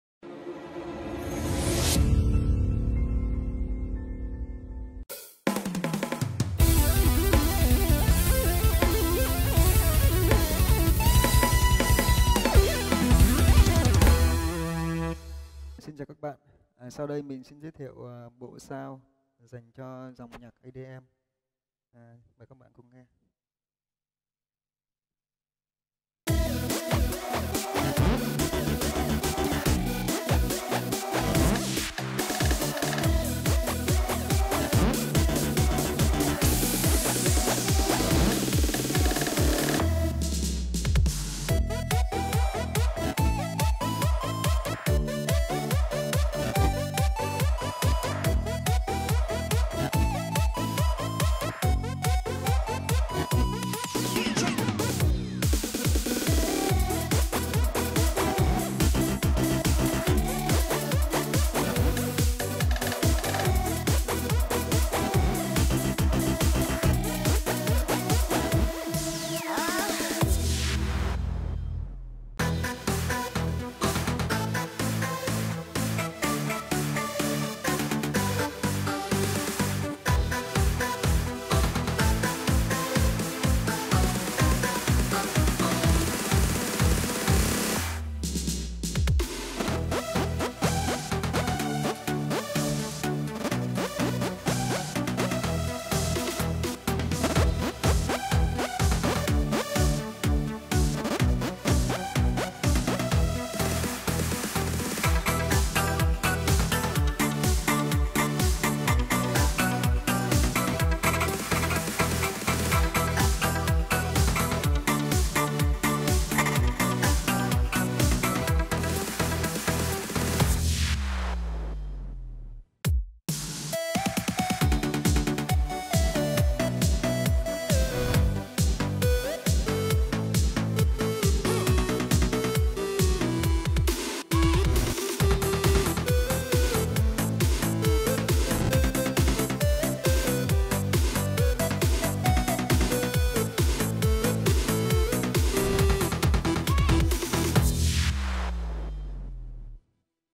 SAMPLE VOICE DJ EDM YAMAHA SX900 & SX700 CỰC CHẤT || Phần 2
Trên đây là video giới thiệu về Sample Yamaha SX700 & SX900. Bên cạnh đó bộ Sample còn hỗ trợ trên đàn Yamaha PSR S775 & PSR S975 .